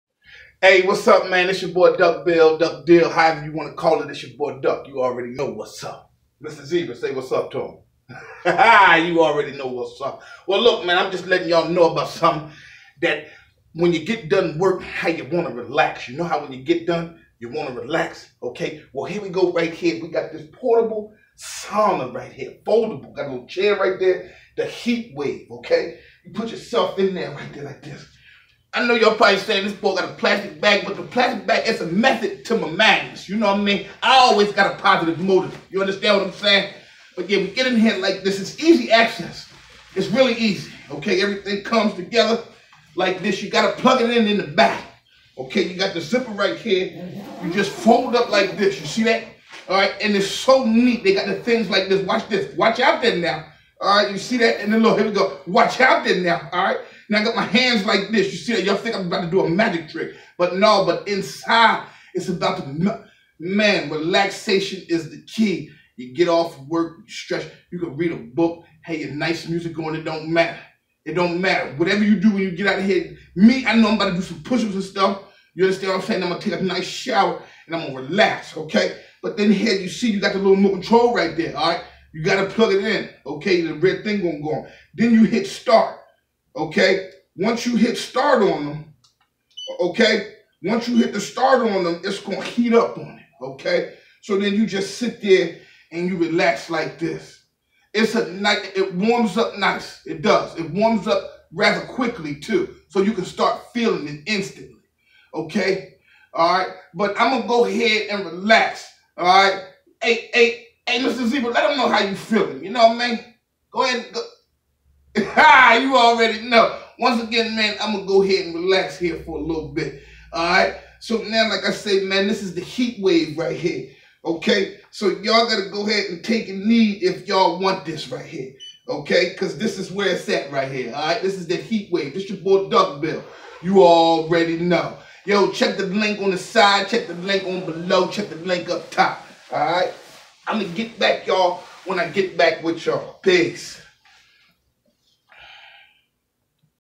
how to use the portable sauna
we all like to relax when we get off of a stressful or hard day of work or at the gym this is for you